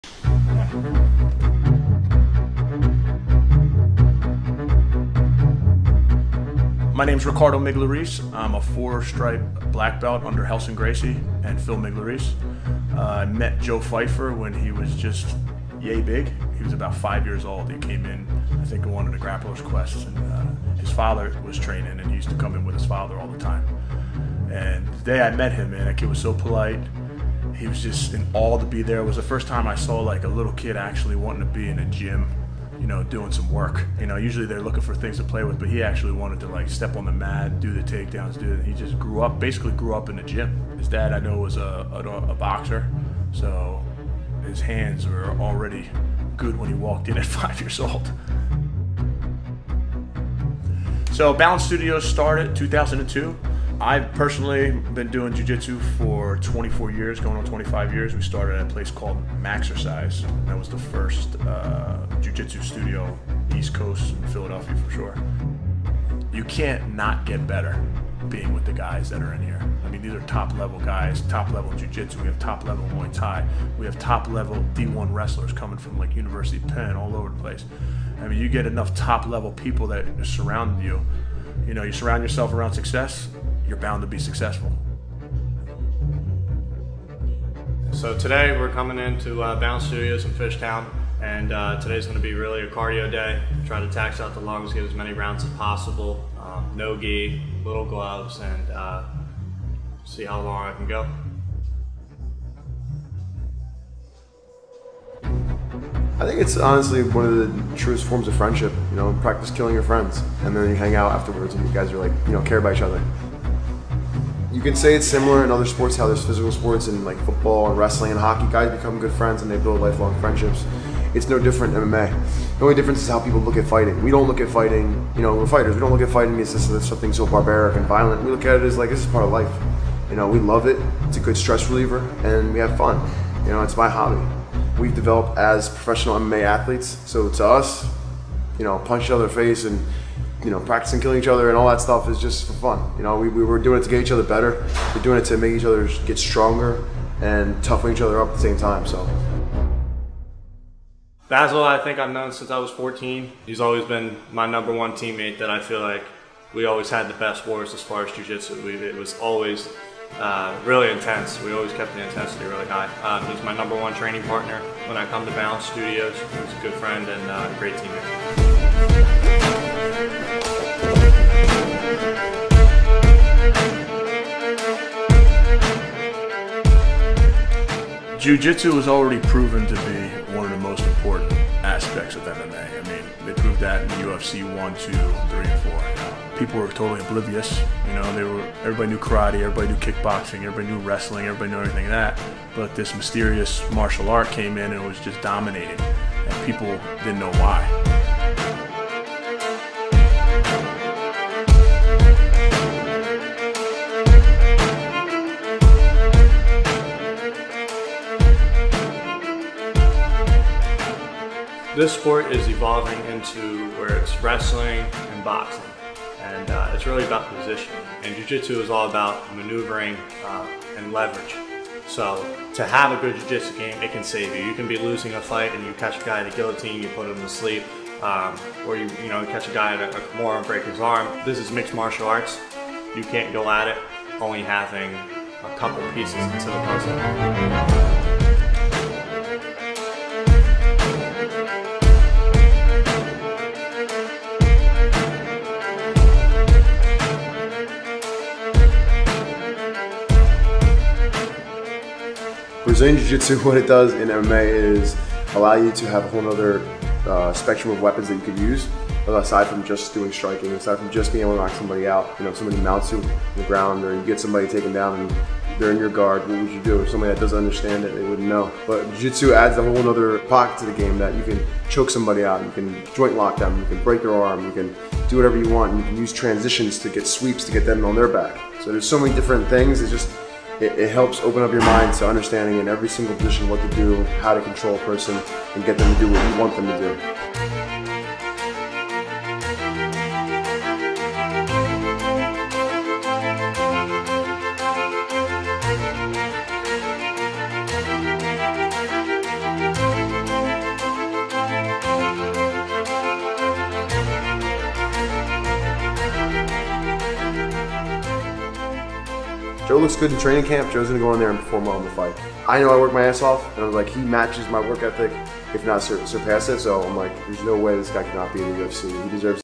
Joe Pyfer journey to UFC
A short documentary of Joe Pyfer’s journey to the UFC. Training at Balance Studios in Philadelphia with the Migliarese brothers. Dana White said it best. “Be like Joe Pyfer”